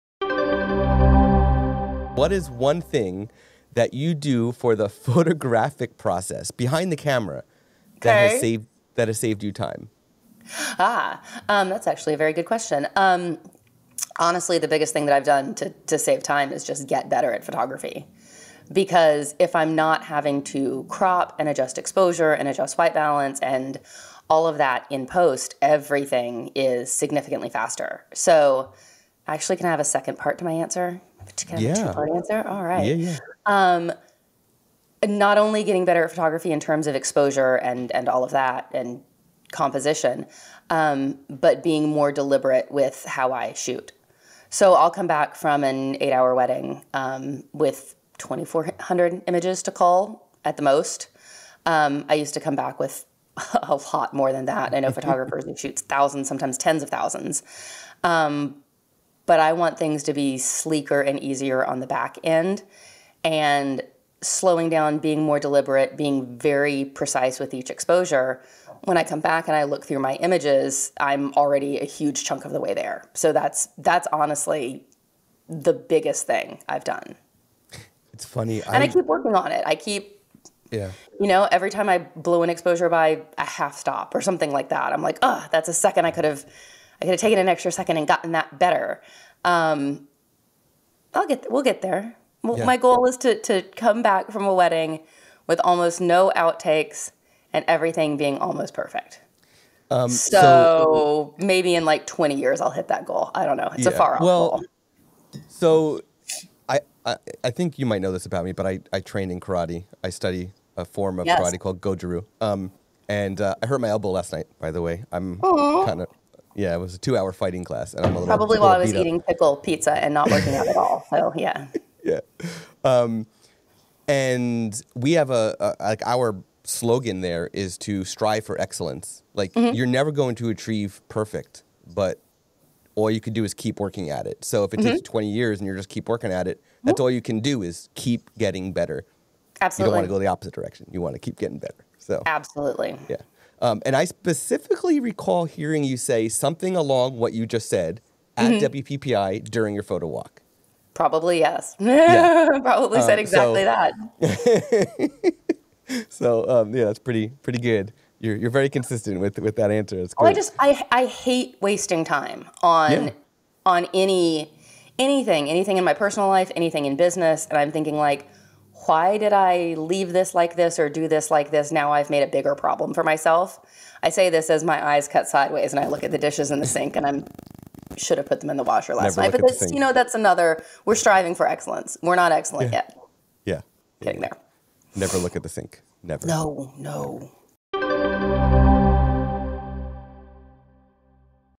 Talking Photographic Workflows with Susan Stripling
Imagen the possibilities.